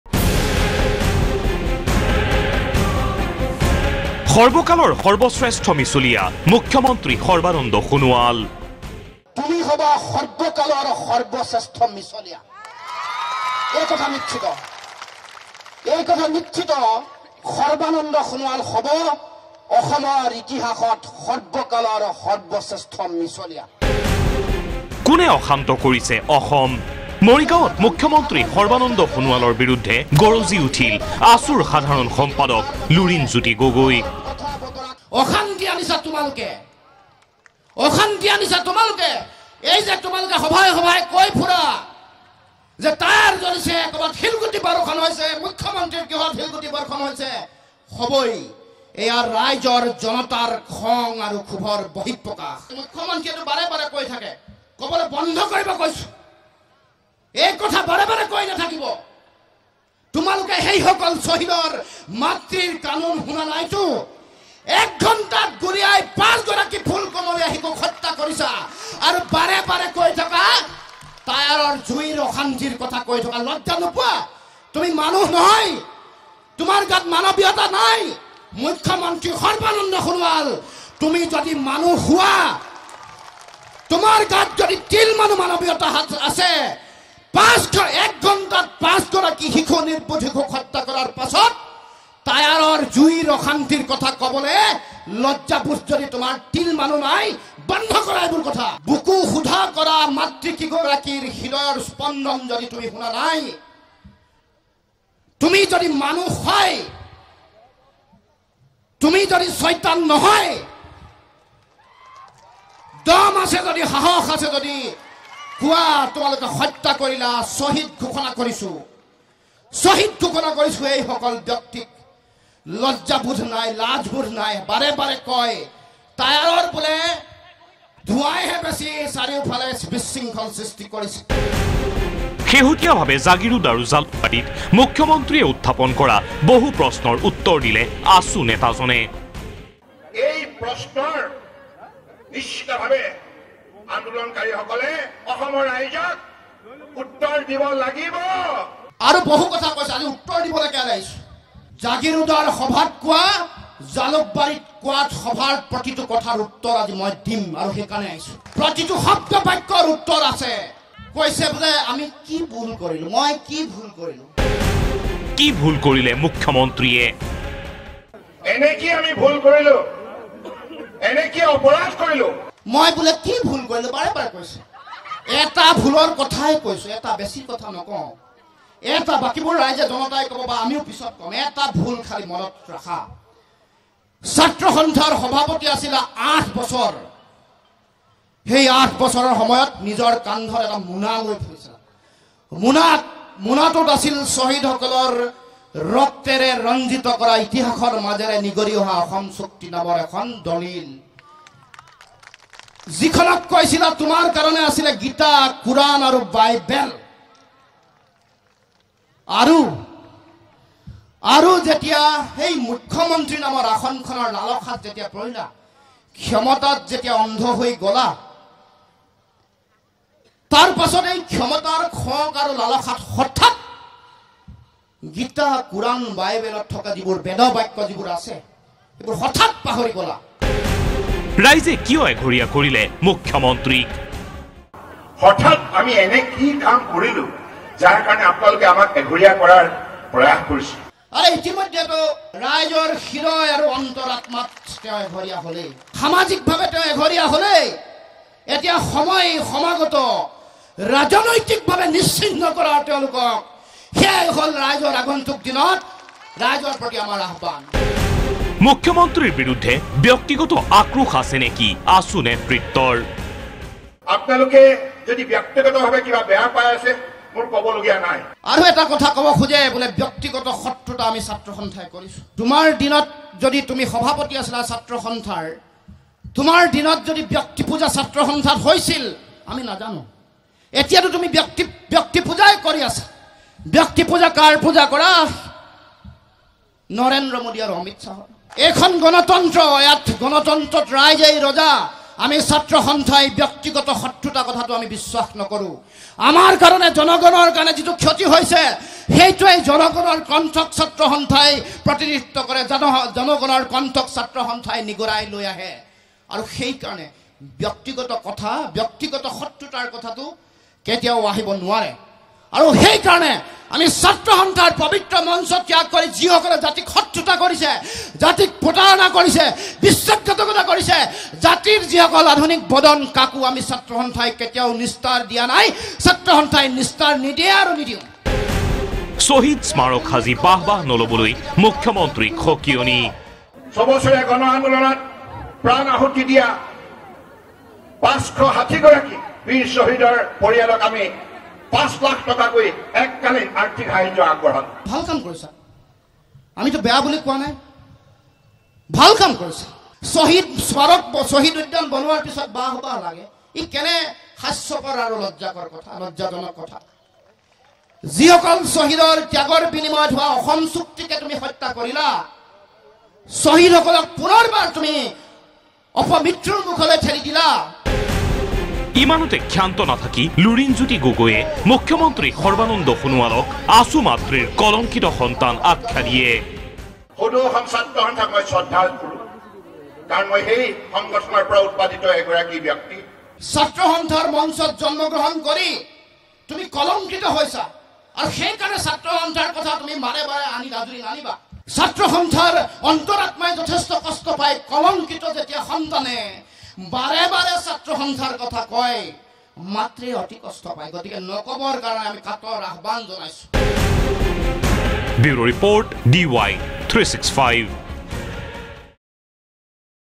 কোনে আশান্ত কৰিছে অসম? মুখ্যমন্ত্ৰীক প্ৰশ্ন লুৰিণজ্যোতি গগৈৰ || AASU Lurinjyoti Gogoi on Assam CM
মুখ্যমন্ত্ৰী সৰ্বানন্দ সোণোৱালক সৰ্বকালৰ সৰ্বশ্ৰেষ্ঠ মিছলীয়া আখ্যা দিলে আছু নেতৃত্বই। লগতে শেহতীয়া ভাৱে জাগীৰোড আৰু জালুকবাৰীত মুখ্যমন্ত্ৰীয়ে উত্থাপন কৰা বহু প্ৰশ্নৰ মৰিগাঁৱৰ বজ্ৰ গৰ্জনৰ মঞ্চৰ পৰা উত্তৰ দিলে আছুৰ সাধাৰণ সম্পাদক লুৰিণজ্যোতি গগৈয়ে।